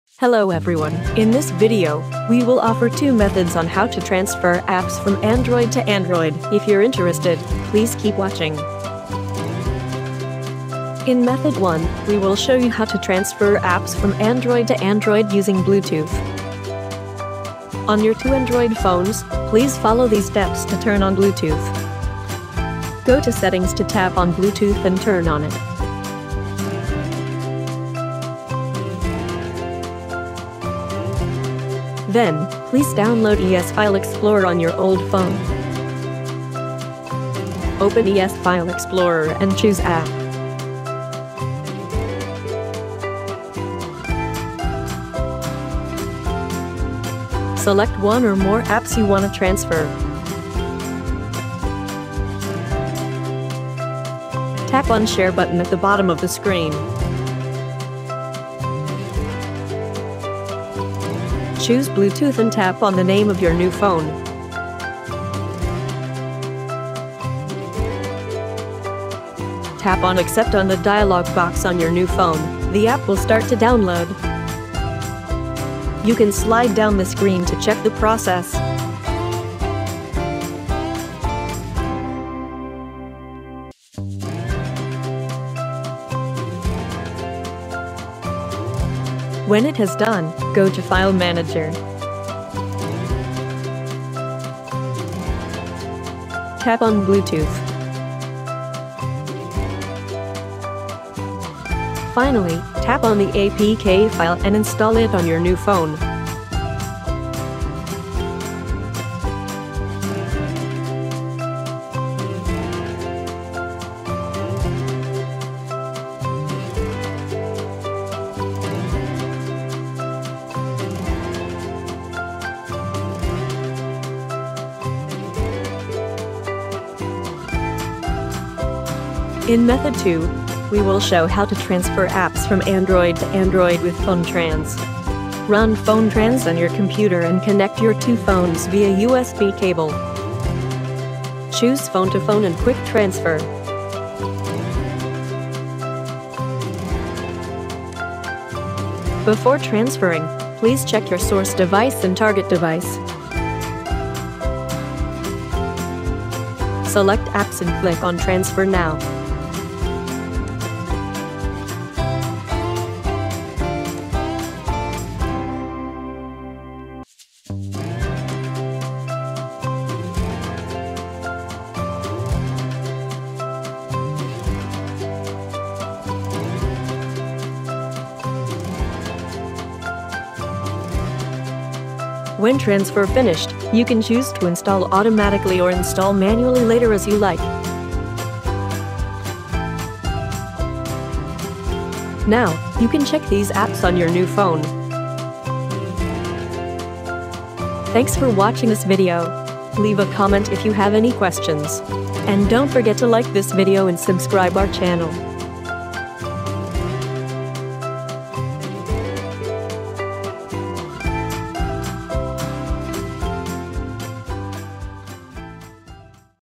[2 Ways] How To Transfer Apps from Android to Android
In this video, we will offer 2 methods to you on how to transfer apps from Android to Android.
00:00 Start
00:19 Method 1. Transfer Apps from Android to Android via Bluetooth
02:13 Method 2. Transfer Apps from Android to Android with PhoneTrans
To get more details on how to transfer apps from Android to Android